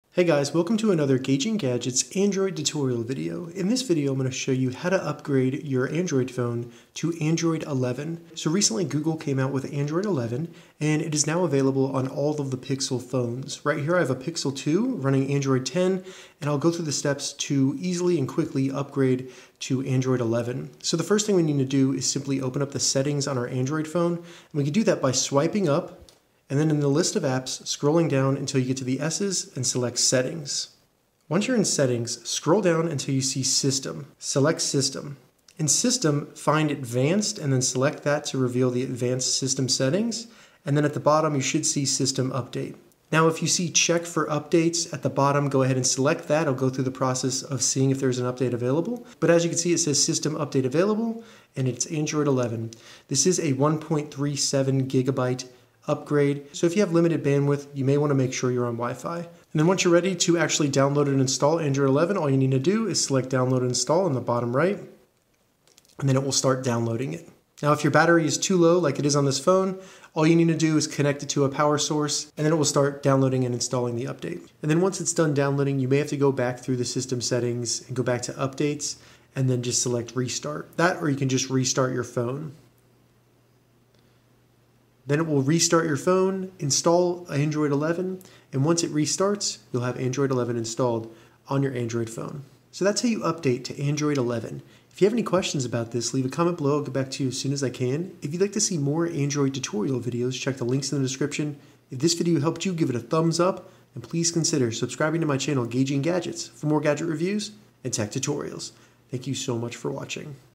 How to Install Android 11 on a Pixel
How to update your Pixel to Android 11?  In this tutorial, I show you how to easily download the new Android 11 on your Android phone.  This does not require any side-loading or a computer. Android 11 includes many new features including a built-in screen recorder.

Note: Android 11 is currently limited to a small number of devices including the Pixel 2, Pixel 2 XL, Pixel 3, Pixel 3 XL, Pixel 3a, Pixel 3a XL, Pixel 4, Pixel 4 XL, Pixel 4a. Check here to see if you phone is compatible with Android

Follow the steps below to update to Android 11:
1. Open the settings on your Android phone.
2. Scroll down and select System.
3. Select Advanced and then System Update.
4. In System Update, first select Check for update.
5. If your device is compatible with Android 11 you will see the option to upgrade to Android 11.
6. Select Download and Install.